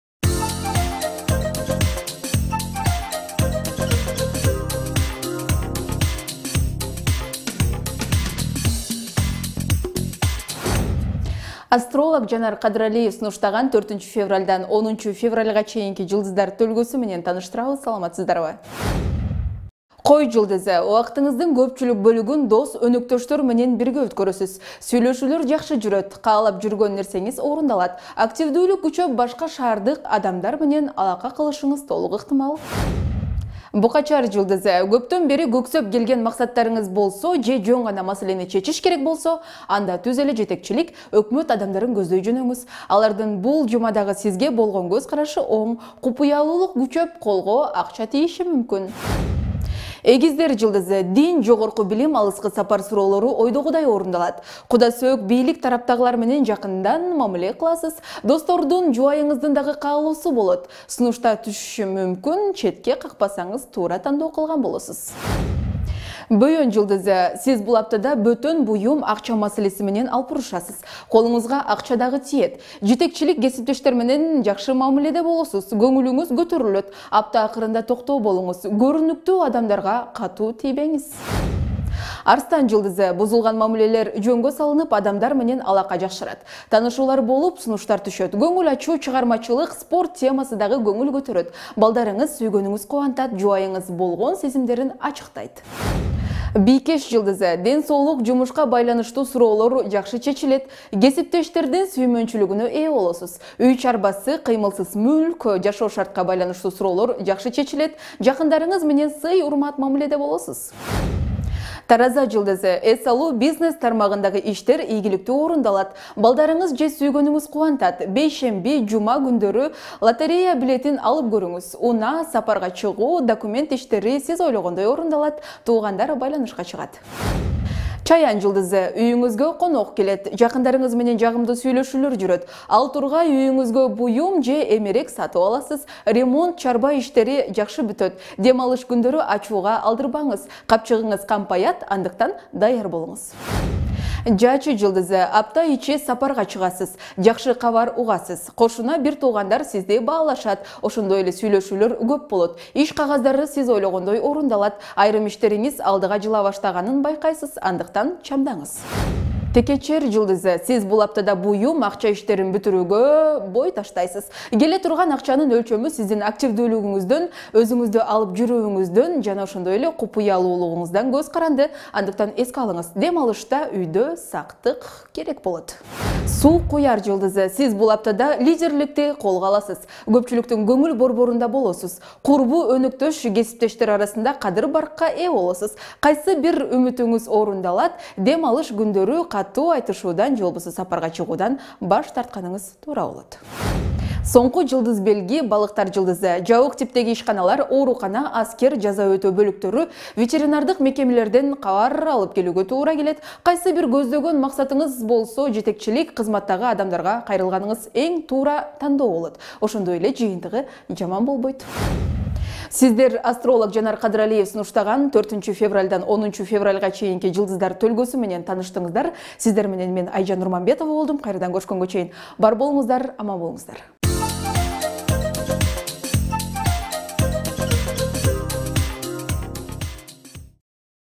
Жылдыз төлгө. 4-10-февралга чейин
Астролог Жанар Кадыралиевдин төлгөсү боюнча 4-10-февраль аралыгында күтүлө турган окуяларды сунуштайбыз. Толугу менен видеодон көрүңүз.